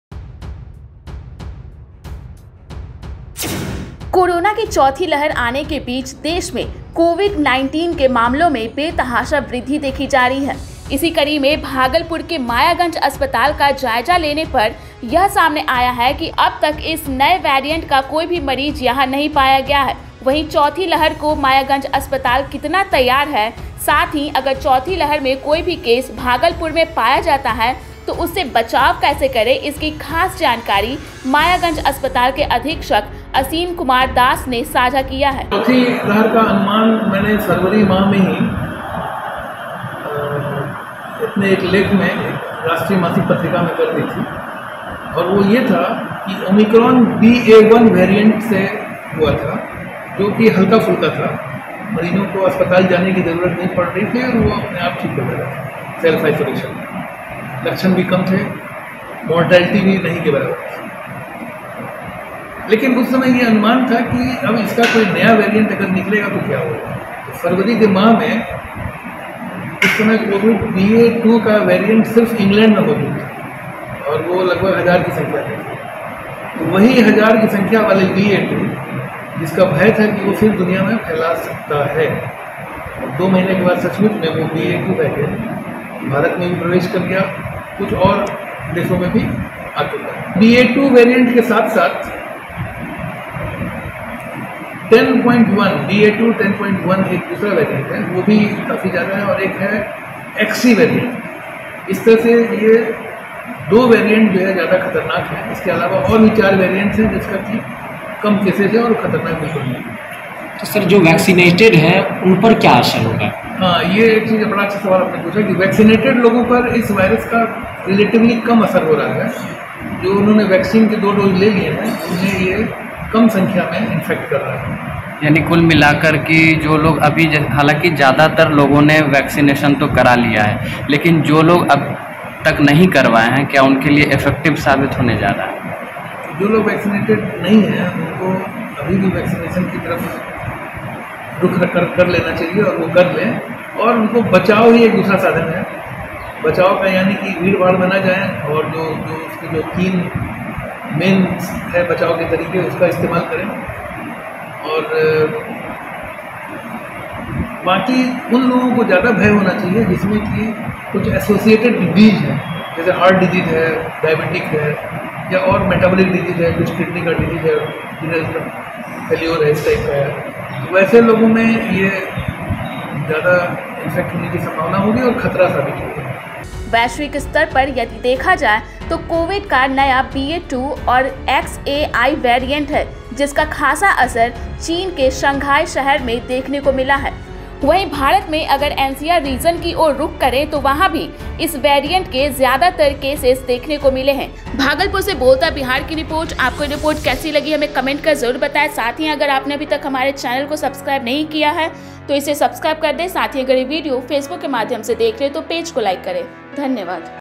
Corona के नए Variant ने दी दस्तक, Bhagalpur में लड़ने के लिए की गयी यह तैयारी...| Bolta Bihar
Corona के नए Variant ने दी दस्तक, Bhagalpur में लड़ने के लिए की गयी यह तैयारी...| Bolta Bihar | Bihar News

देखें वीडियो, देश में बढ़ रहे कोरोना के मामले, भागलपुर नए वेरिएंट से लड़ने के लिए तैयार.